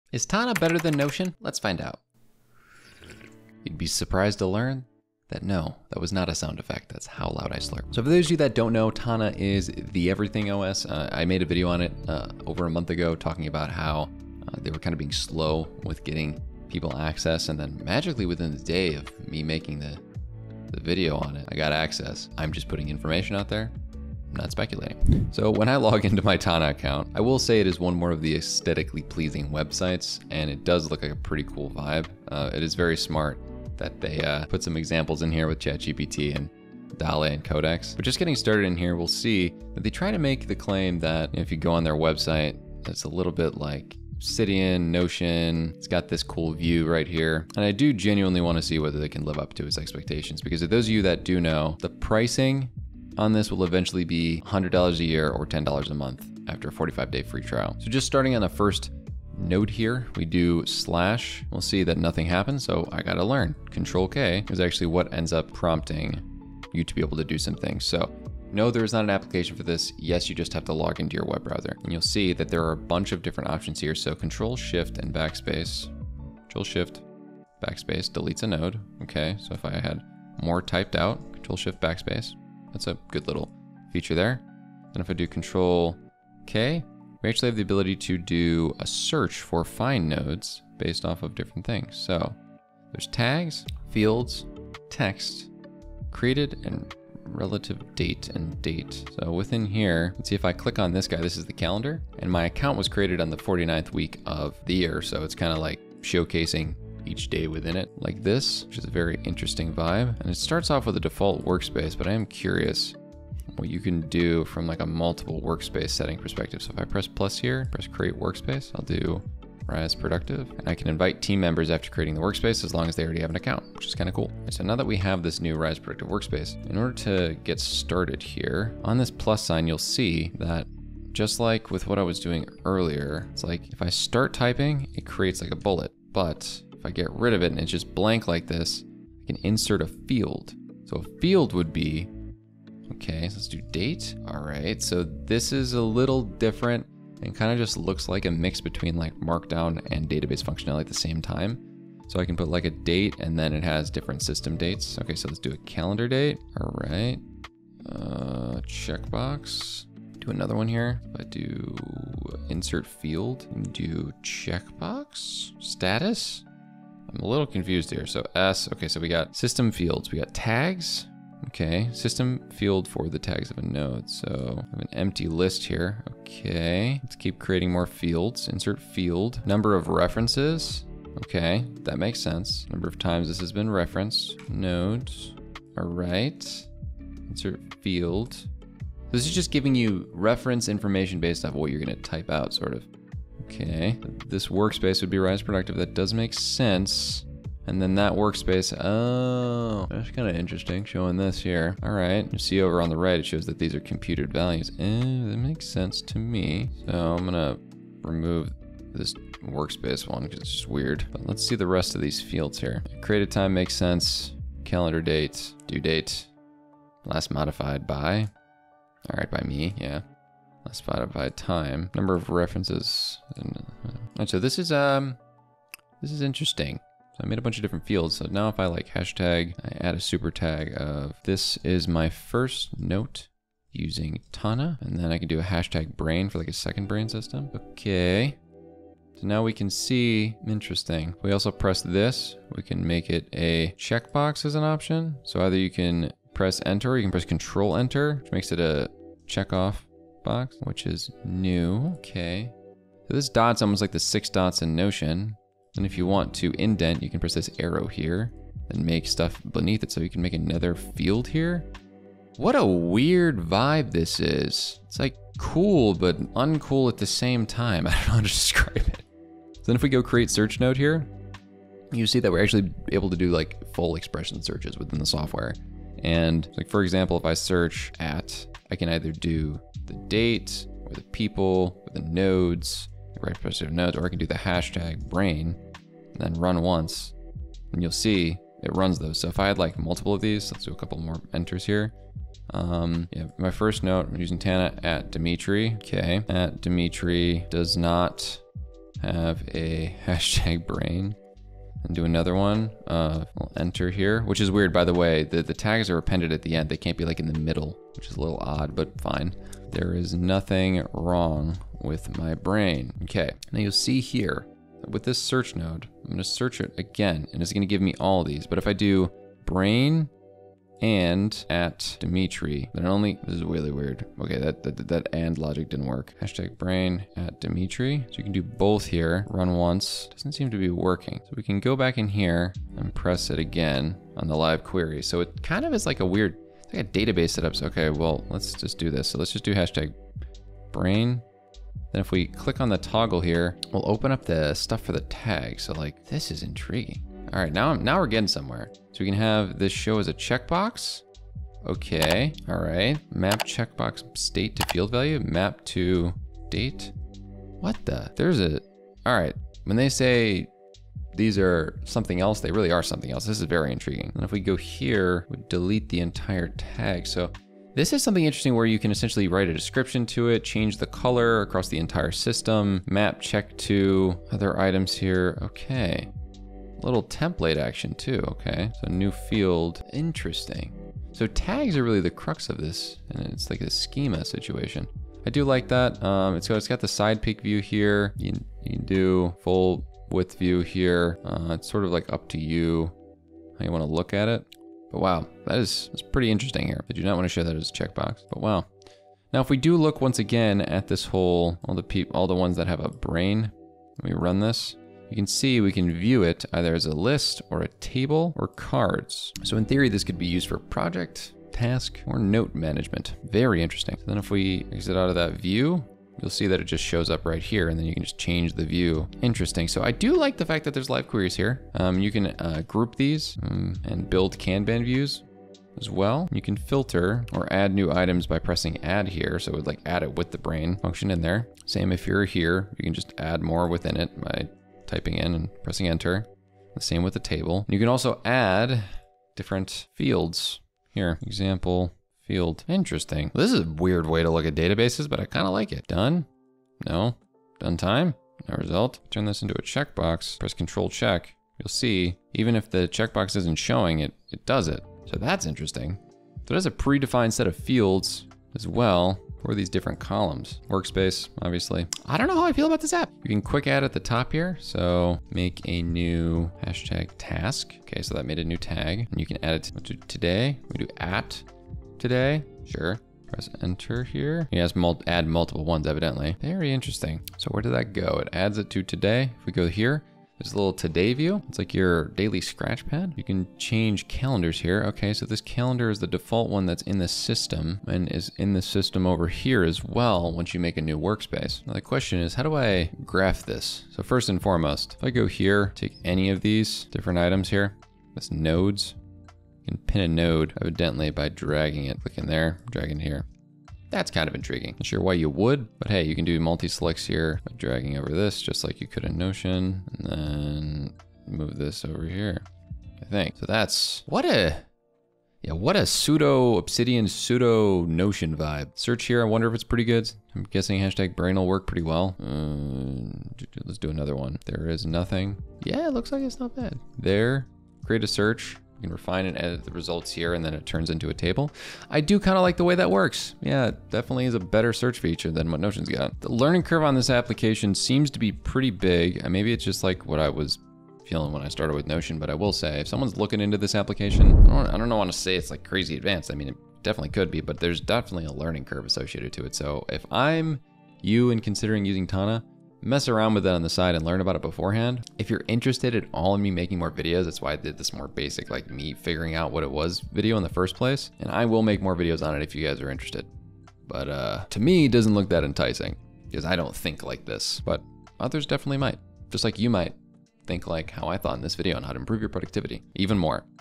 Tana - Better Than Notion?!
In this video, I'll be comparing Tana and Notion to determine which one is superior. I recently had the opportunity to try Tana and it certainly appears promising. It has a similar aesthetic to Notion, but we'll see if it can live up to expectations.

⏰ TIMESTAMPS:
0:00 - Is Tana OS Better Than Notion?
0:26 - Aesthetic
0:56 - Pricing
1:00 - Tana's Features and interfacr